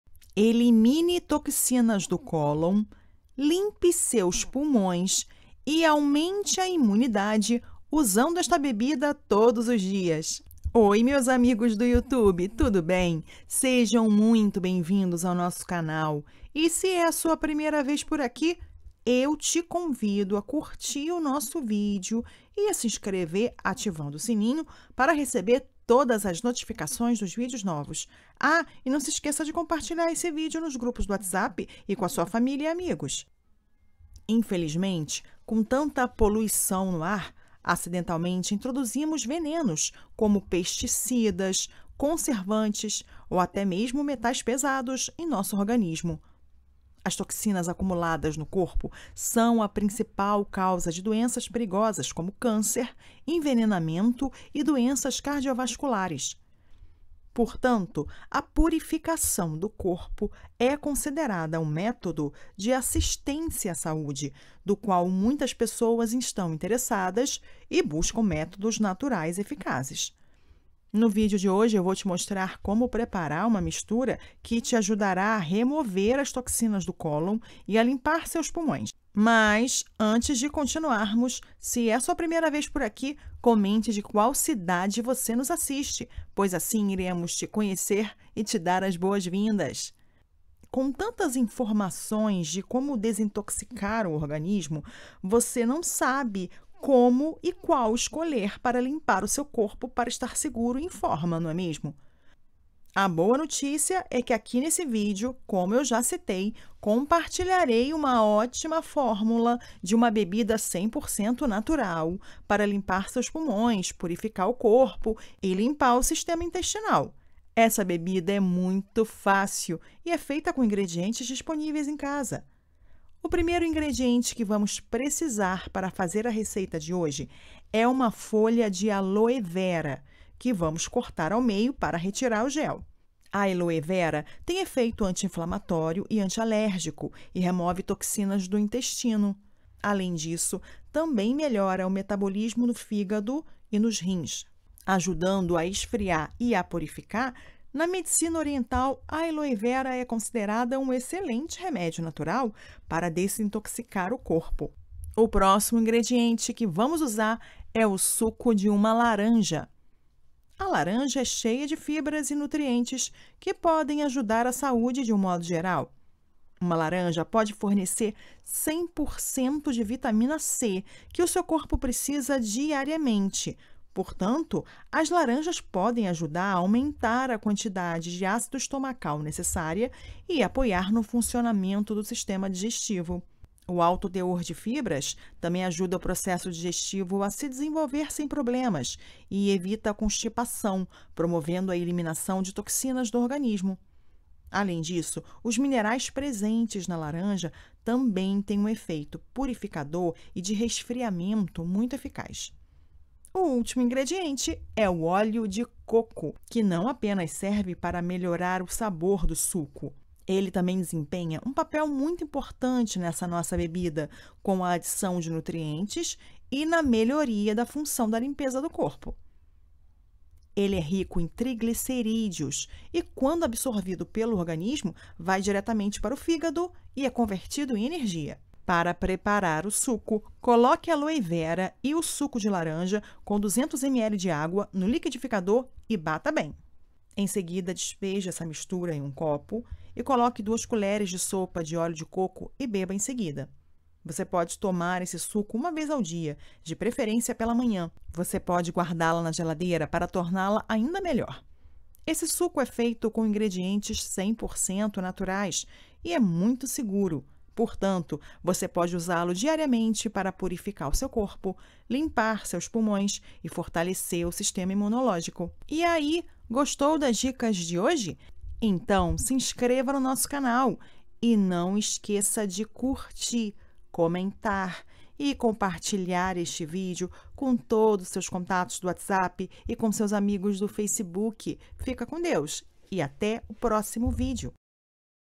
Elimine Toxinas do Cólon, Limpe Seus Pulmões e Aumenta a Imunidade Usando Esta Bebida Todos os Dias!
Infelizmente com tanta poluição no ar  acidentalmente introduzimos venenos como pesticidas , conservantes ou até mesmo metais pesados em nosso organismo.

As toxinas acumuladas no corpo são a principal causa de doenças perigosas como câncer , envenenamento e doenças cardiovasculares.
Portanto, a purificação do corpo é considerado um método de assistência  à saúde do qual muitas pessoas estão interessadas e buscam métodos naturais eficazes.

No vídeo de hoje eu vou te mostrar como preparar uma mistura que te ajudará a remover as toxinas do cólon e a limpar seus pulmões.